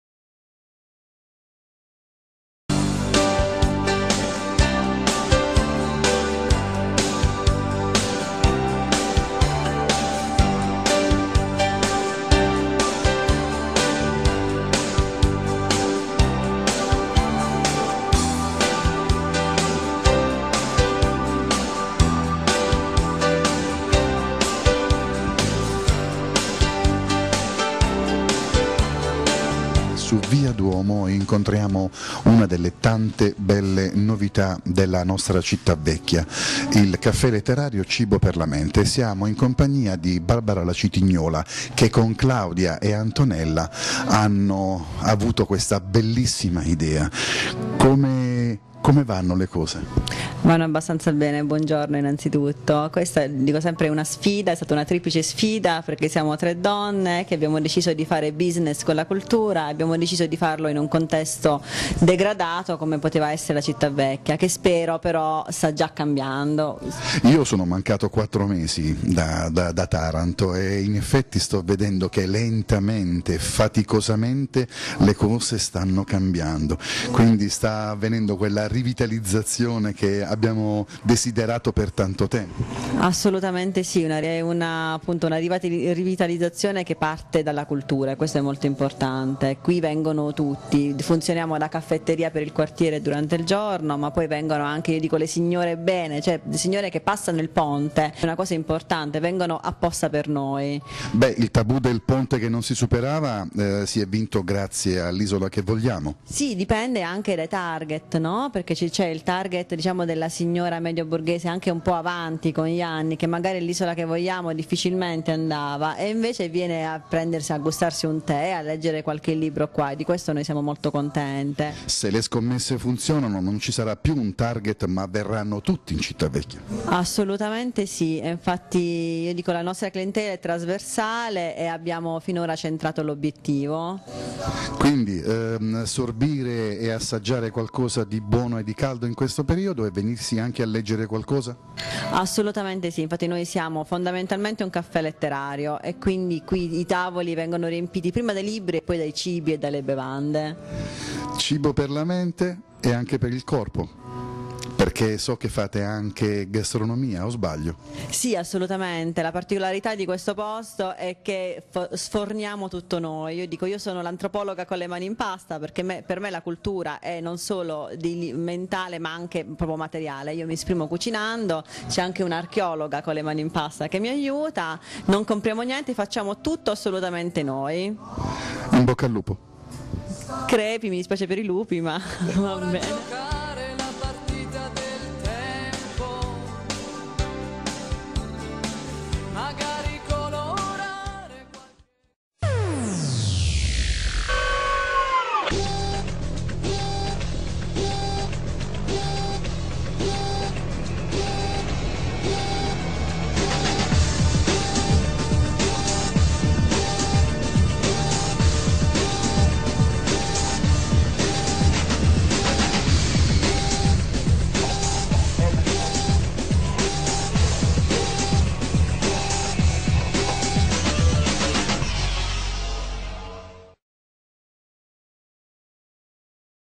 "CIBO PER LA MENTE" IL CAFFE' LETTERARIO NEL BORGO ANTICO DI TARANTO
Antonella , Barbara e Claudia . A , B , C . L'abbeceddario del buon vivere . I primi rudimenti del sapere . Sapere  declinato in  tutte le sue forme possibili . Arte , letteratura , musica , pittura e poesia . E ancora cultura gastronomica e del buon bere . Una vera e propria fucina delle idee , quella immaginata e realizzata da queste tre belle figliole nel cuore della città antica . Una sosta rigenerante e vivificante . Una gioia per gli occhi , il palato e la mente . Un uragano di stimoli positivi che non lascia assolutamente indifferenti . Un crogiuolo di arti e passioni per parlare e discettare della vita , nelle sue declinazioni del buono e del bello . Da visitare necessariamente .